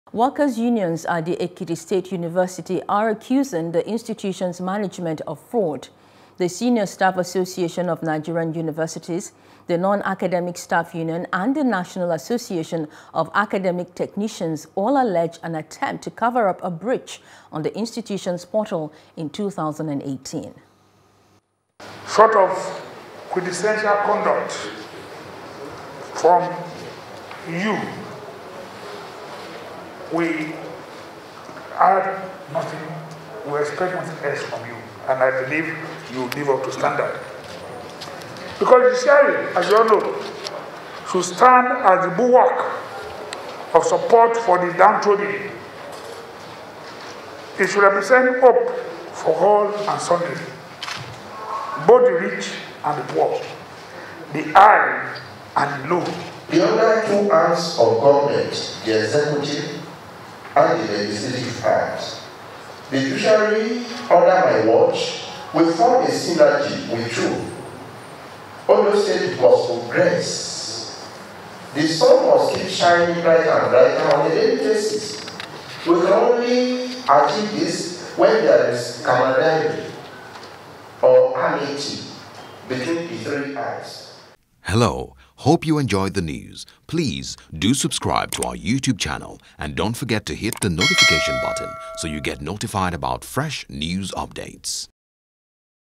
Ekiti State University: Three Workers’ Unions Accuse Institution’s Management Of Fraud | NEWS
Workers' unions at the Ekiti state university are accusing the institution’s management of fraud.